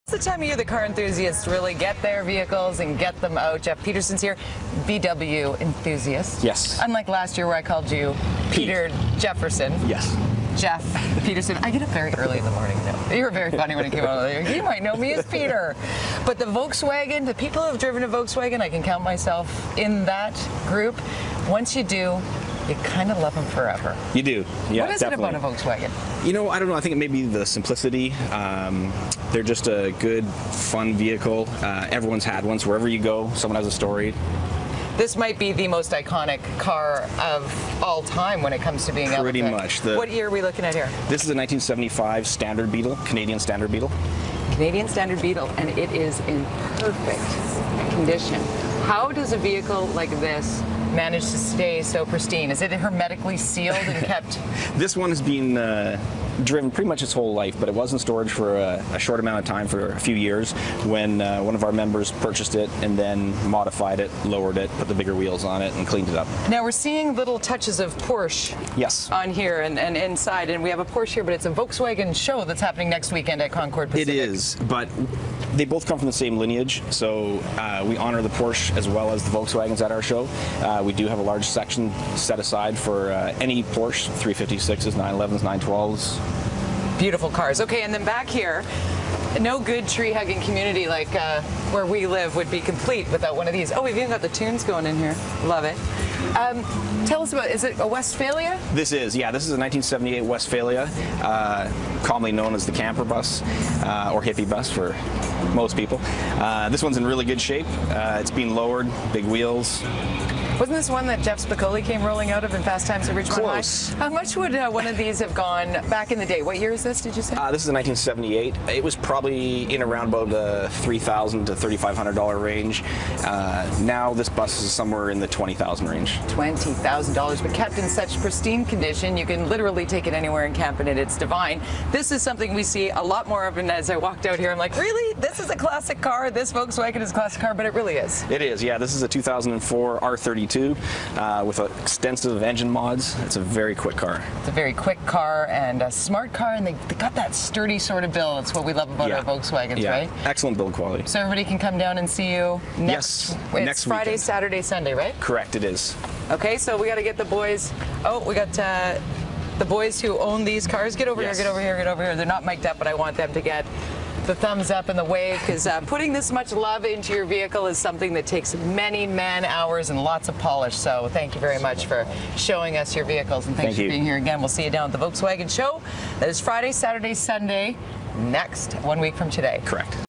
Great Canadian VW Show
Vintage bug? Latest Cabrio? VW fans are buffing their prized possessions gearing up for a great weekend. We get a preview of the Great Canadian VW Show!